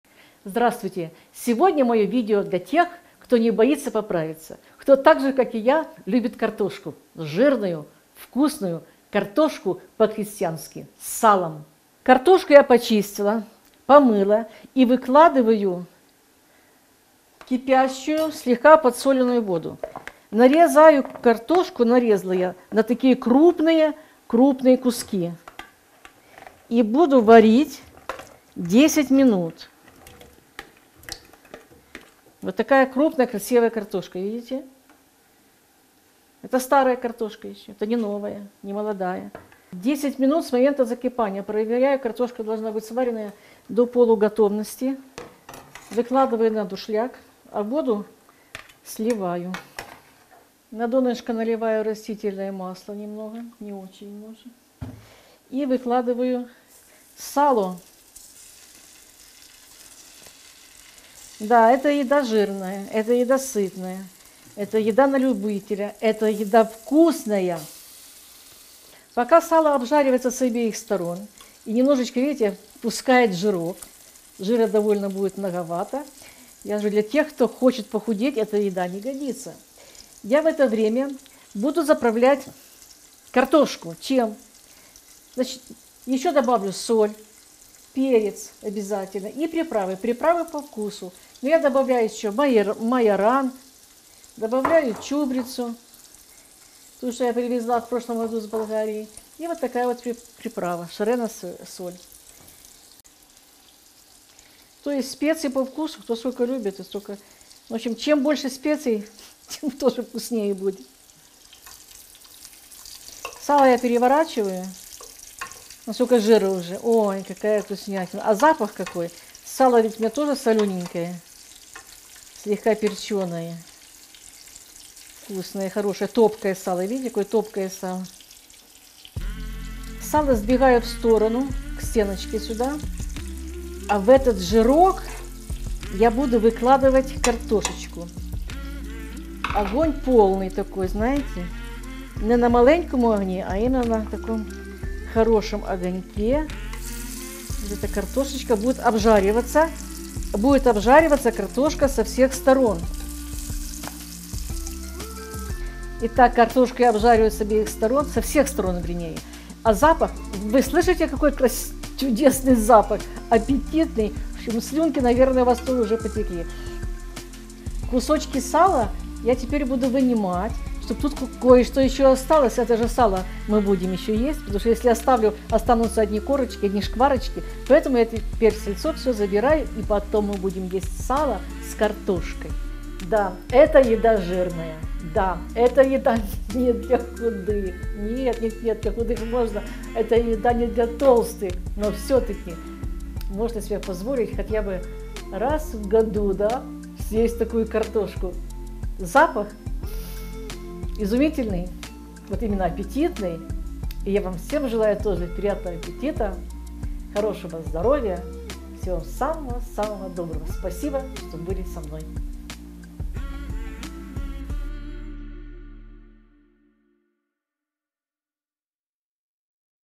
Очень Вкусная Жареная Картошка по Деревенски с Салом
Жаренная Картошка по Крестьянски с Салом- простые и добрые советы от меня. С Вами Папсуева Ольга. Пишите отзывы они очень важны для меня